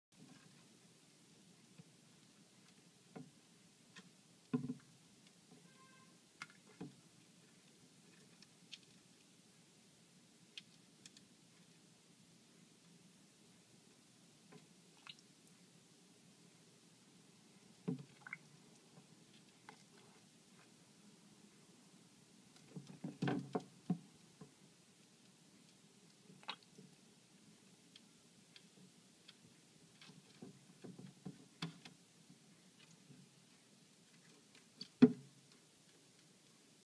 Little Turtles Fight Over Cockroach
They both grew up to be ten inches long.